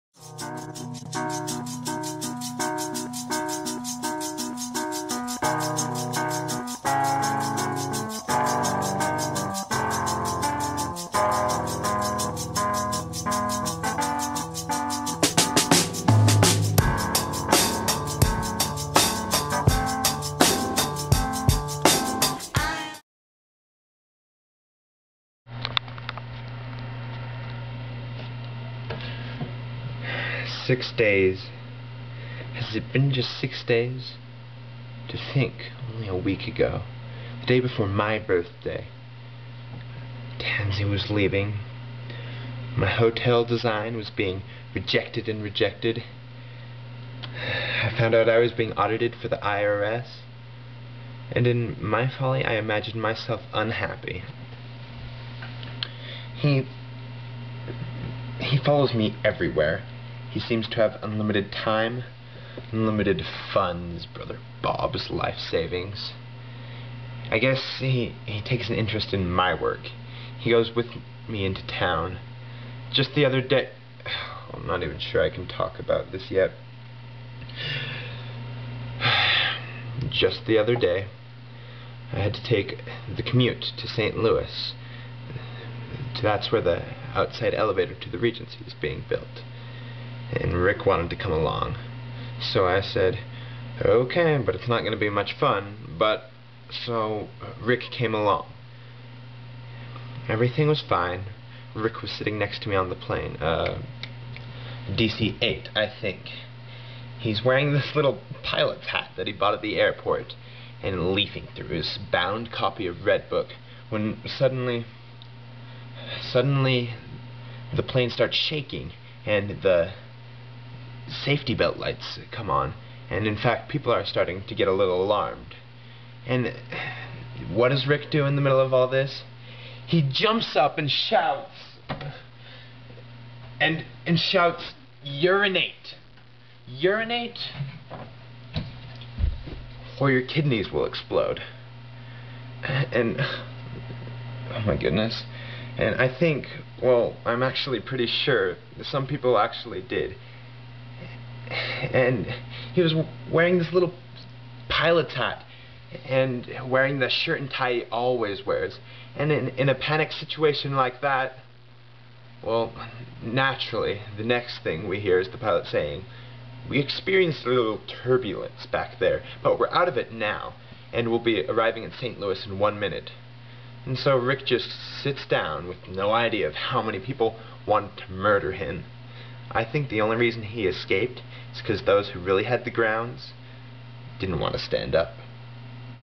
The Nerd - Larry Shue
Act II Scene I: Monologue performed by Joshua James Hardy